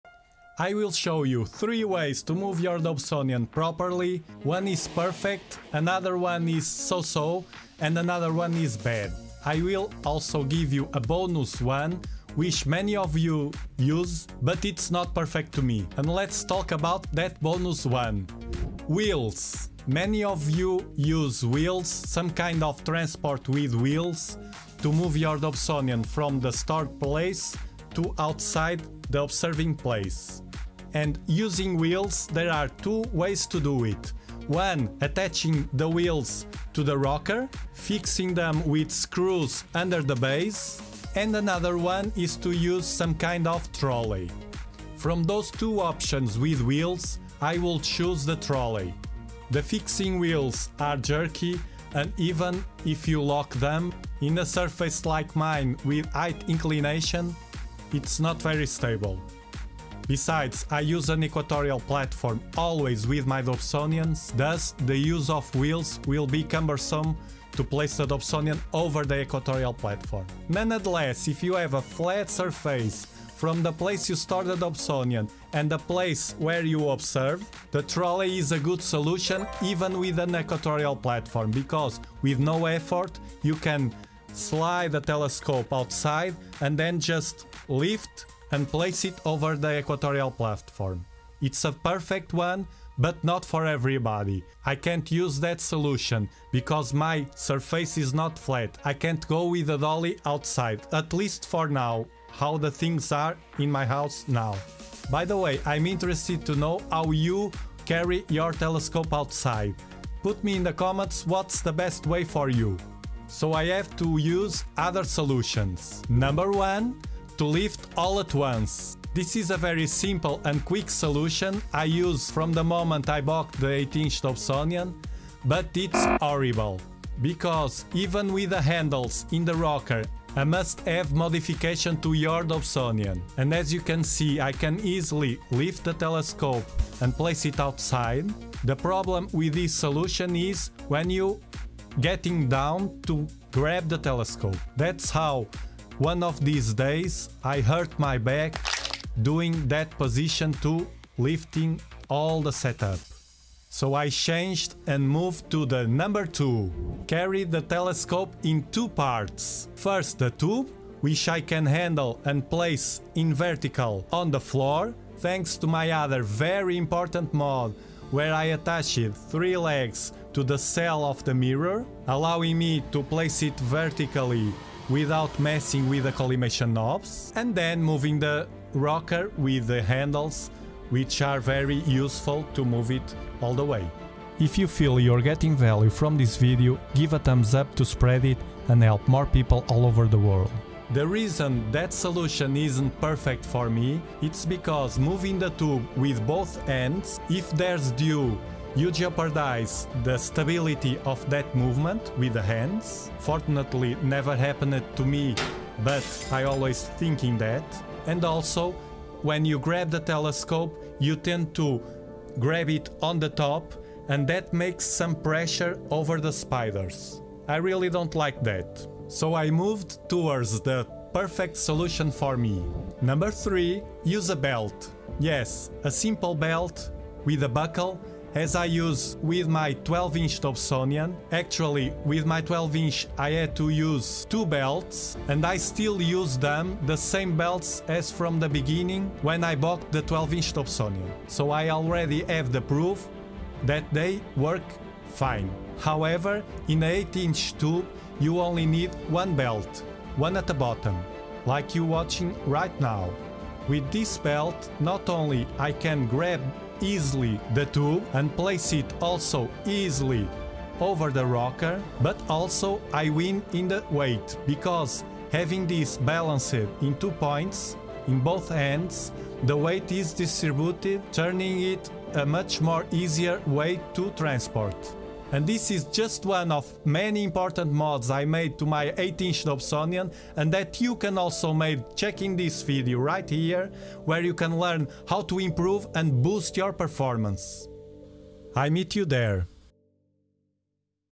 NEVER Do This With Your 8" Dobsonian Telescope
3 ways to move your 8 inch Dobsonian Telescope, 1 of them is perfect for me and you can do it with a simple and inexpensive mode. No more pain in the back for me lifting the Telescope.